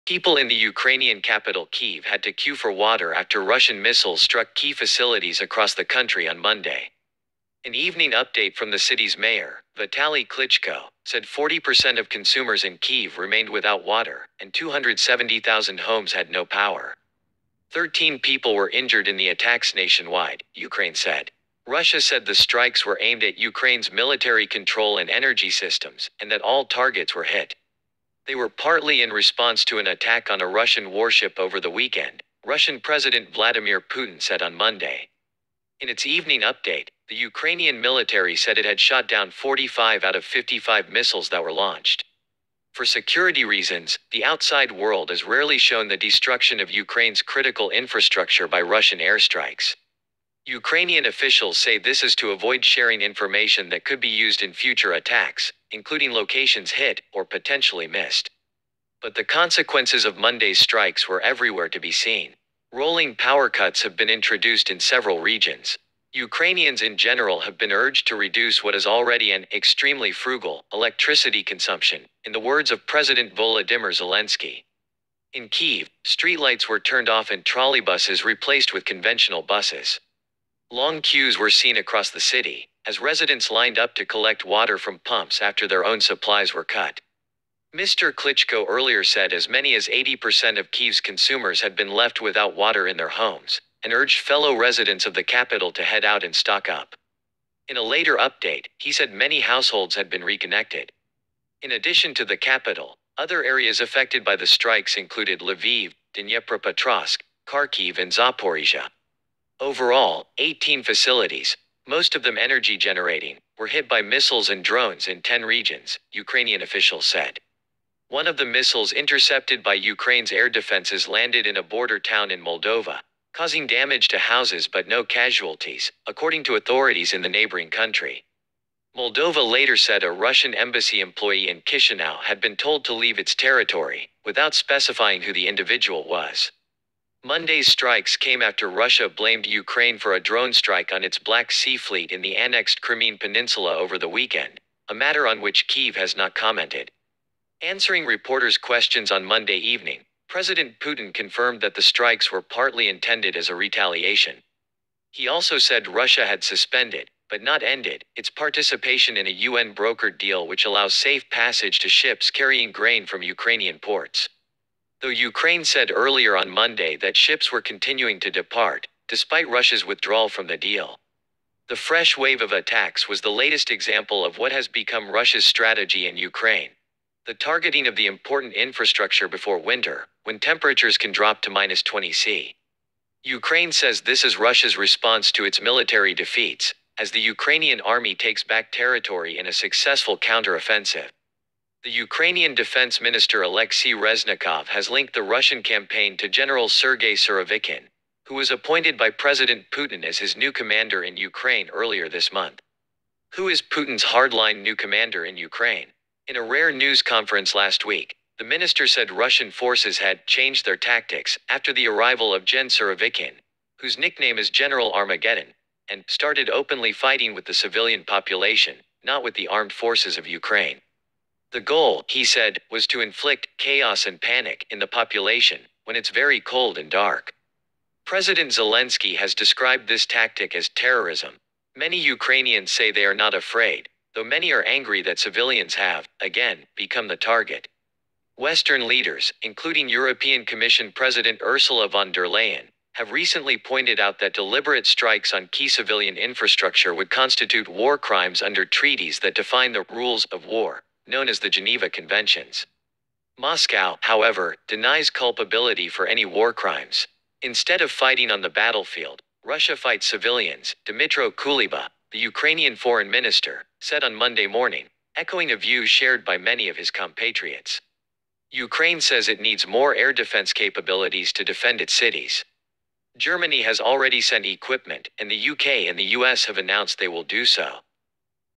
Ukraine war: Kyiv locals queue for water after Russian strikes
Hello friends, Today I present to you the new world news Ukraine war: Kyiv locals queue for water after Russian strikes video.